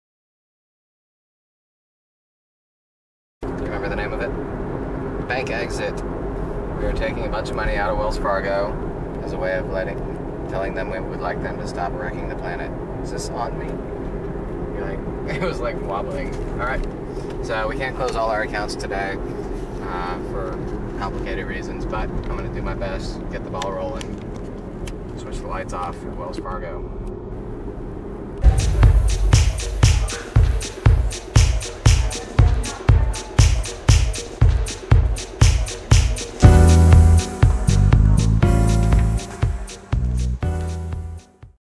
My BankExit from Wells Fargo
Say goodbye to a "High Value Customer" and say NoDAPL !
So my closing title card is messed up, here's what it says:
Removed $15,000
Also noticed that the screen said
"High Value Customer" when they
pulled my account up, so I told
the guy why I was leaving and left
a BankExit note for him to research.